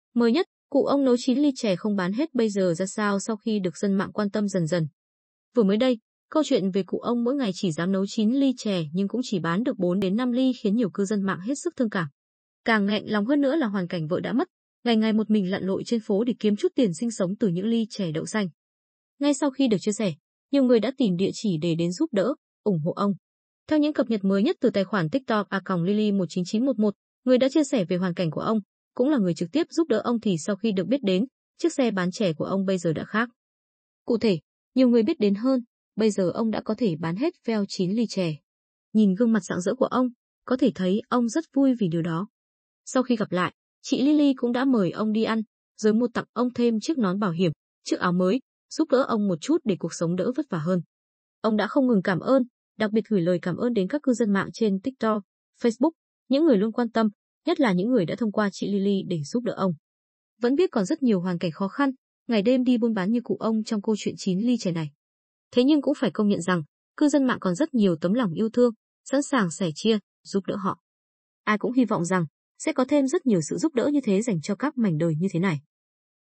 MỚI NHẤT: Cụ ông nấu 9 ly chè không bán hết bây giờ ra sao
Vừa mới đây, câu chuyện về cụ ông mỗi ngày chỉ dám nấu 9 ly chè nhưng cũng chỉ bán được 4 - 5 ly khiến nhiều cư dân mạng hết sức thương cảm. Càng nghẹn lòng hơn nữa là hoàn cảnh vợ đã mất, ngày ngày 1 mình lặn lội trên phố để kiếm chút tiền sinh sống từ những ly chè đậu xanh.

Ngay sau khi được chia sẻ, nhiều người đã tìm địa chỉ để đến giúp đỡ, ủng hộ ông.

Theo những cập nhật mới nhất từ tài khoản TikTok @lyly19911, người đã chia sẻ về hoàn cảnh của ông, cũng là người trực tiếp giúp đỡ ông thì sau khi được biết đến, chiếc xe bán chè của ông bây giờ đã khác.